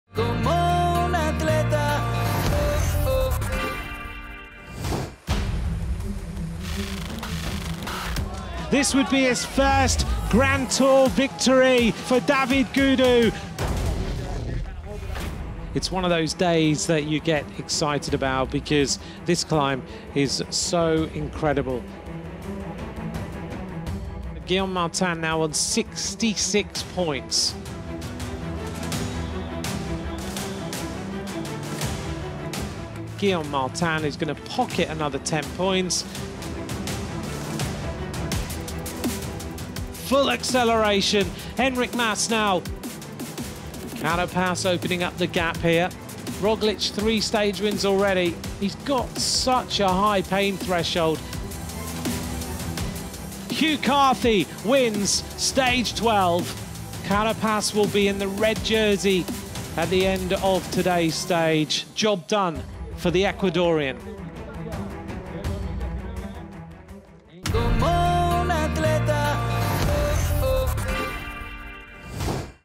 Resumen Flash / Flash Summary - Etapa 12 | La Vuelta 20
La Vuelta 20 tendrá lugar del martes 20 de Octubre al domingo 8 de Noviembre, incluirá 18 etapas para esta 75ª edición.

Etapa 12 - (La Pola Llaviana/Pola de Laviana / Alto de l'Angliru)

Hashtag Oficial